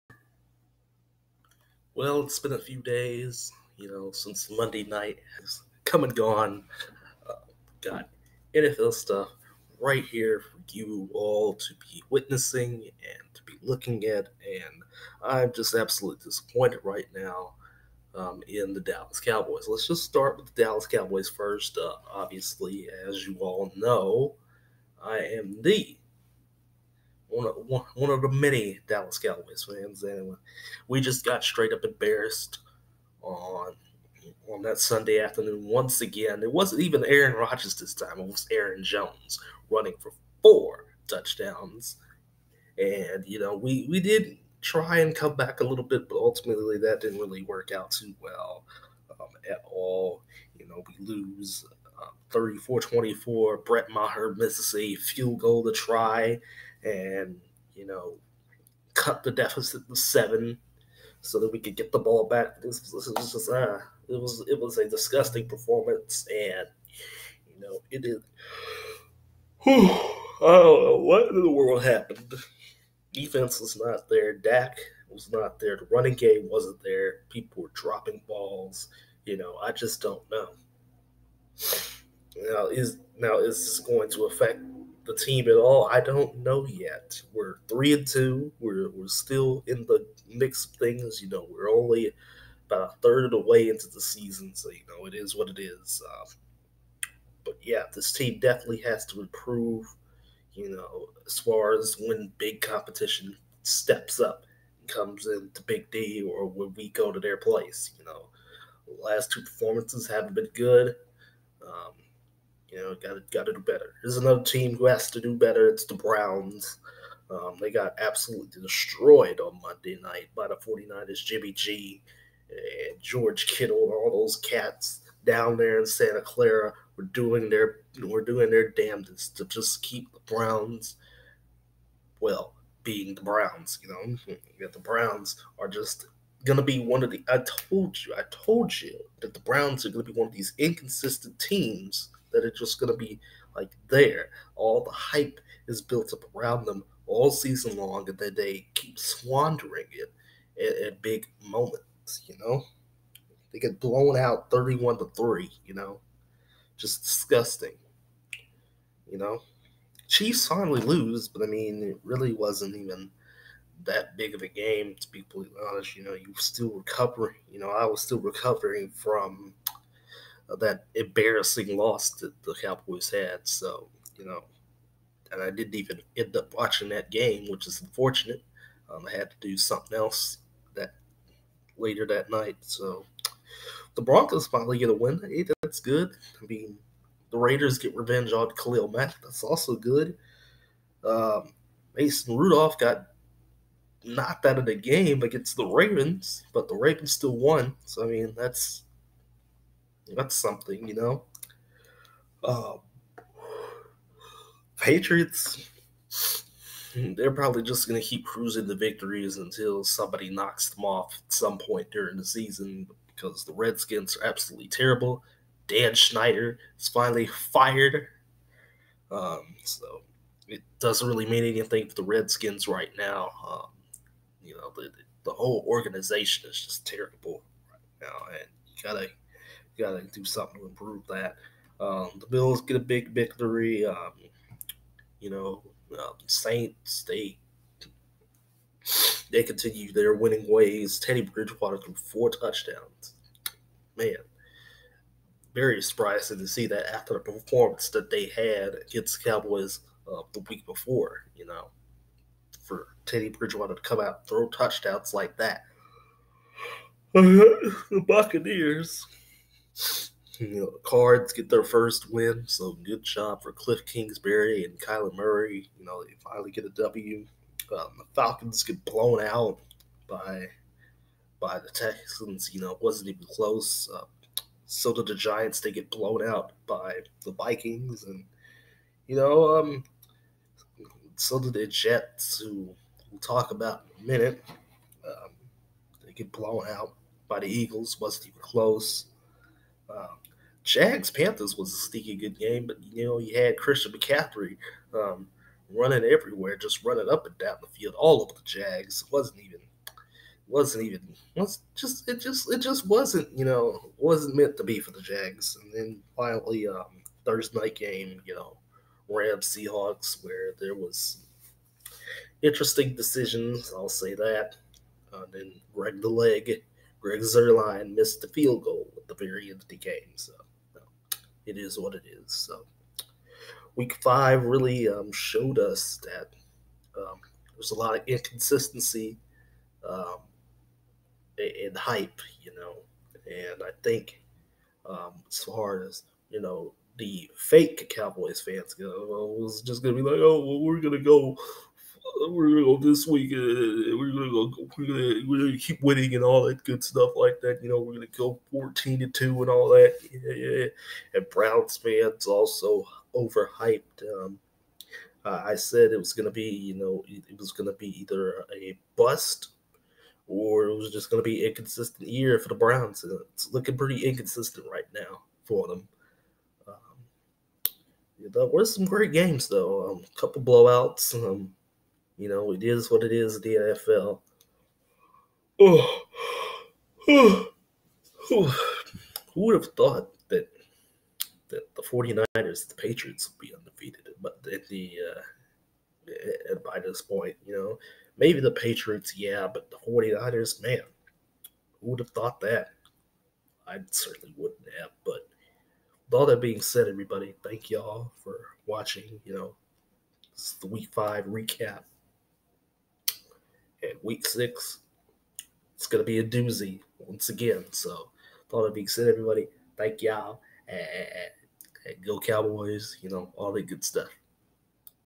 NFL Week 5 Recap: Are the Browns and Cowboys Overhyped Teams?
I hate that I missed the SNF game tho